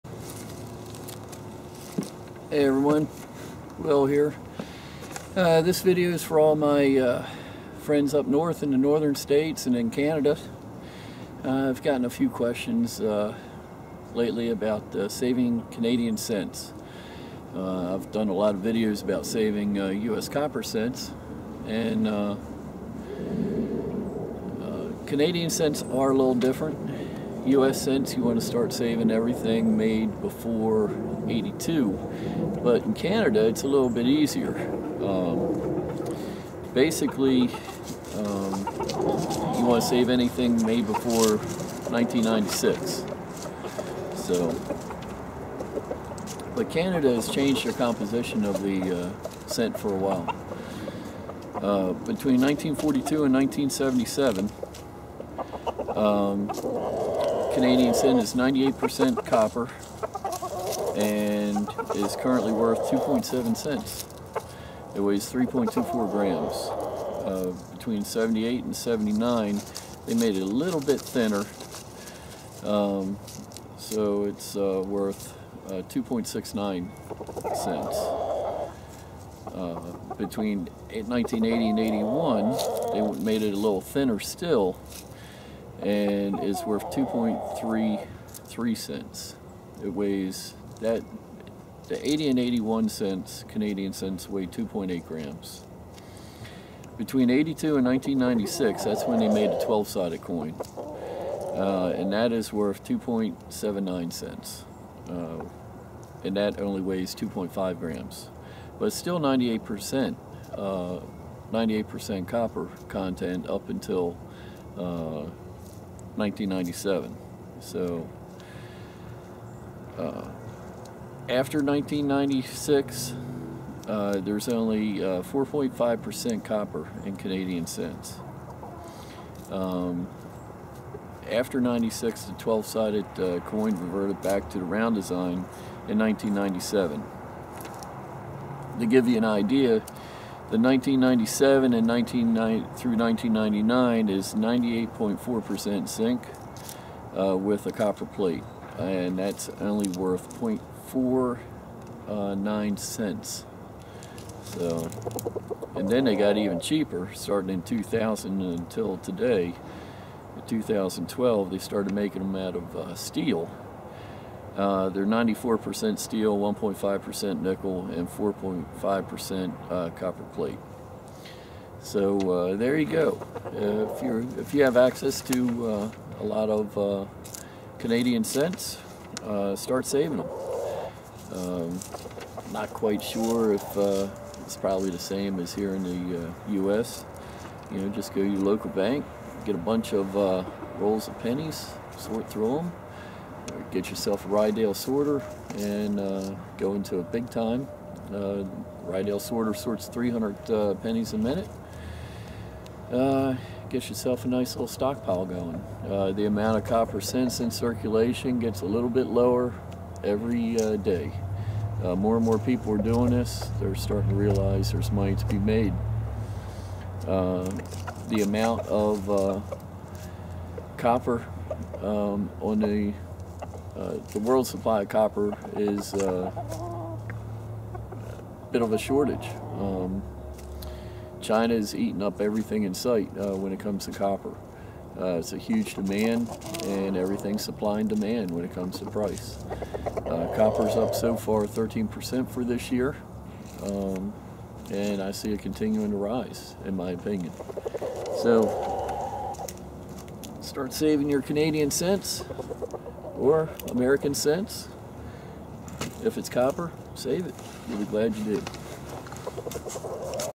Which Canadian Penny Years to Save for Copper
For those of you that have access to Canadian pennies, here is what to look for to save the copper cents. - Link to my terminal99 eBay store!